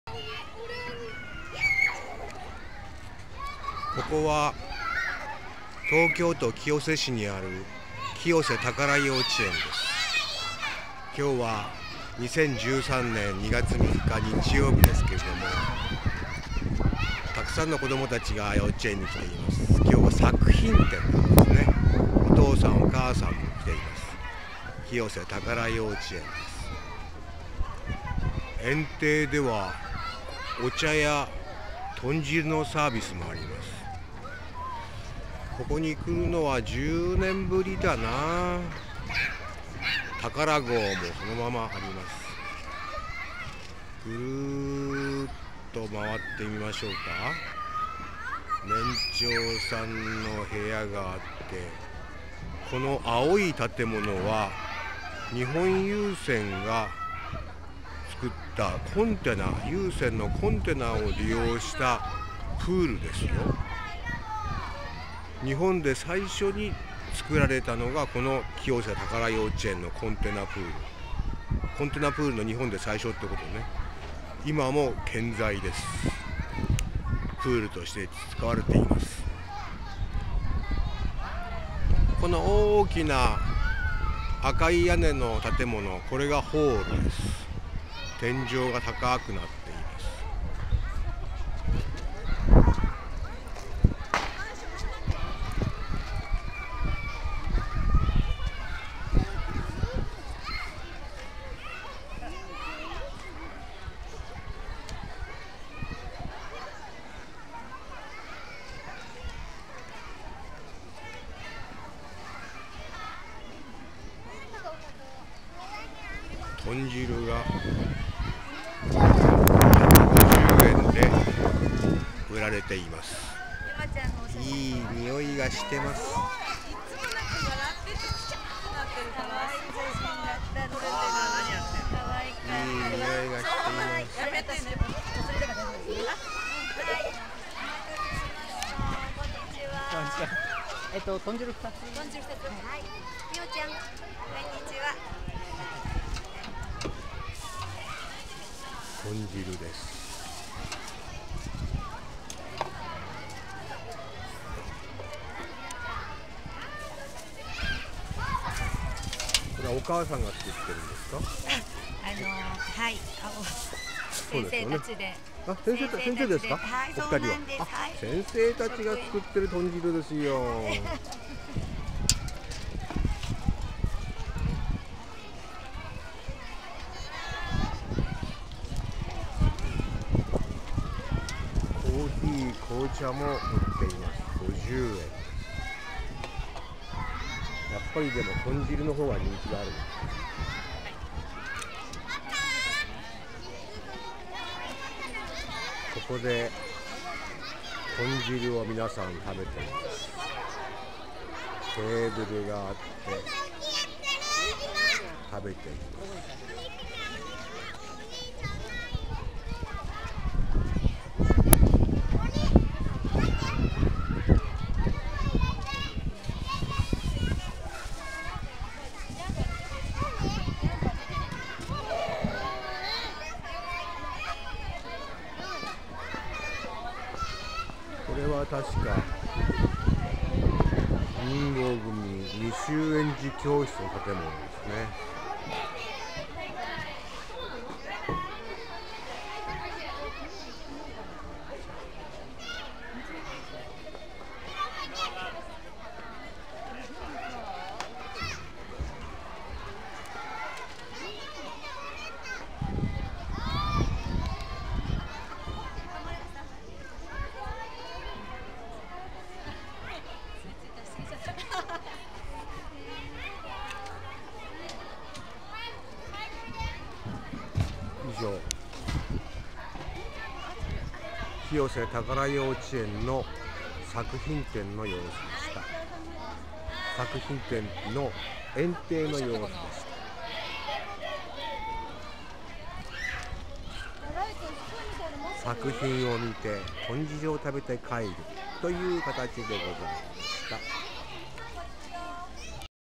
130203清瀬たから幼稚園の作品展「園庭の様子」
2013年2月3日(日)に行われた東京都清瀬市・清瀬たから幼稚園(粕谷勇理事長＆園長)の作品展に行ってきました。まずは園庭の様子です。作品展を見終わった親子が、先生方が作ってくれた豚汁を食べたり、紅茶・珈琲を飲んで一休み。これが作品展のちょっとした楽しみになっているようでもあります。同園には日本郵船のコンテナを利用した室内温水プールがあります。1985年頃に設置された日本第1号ですが、今も健在です。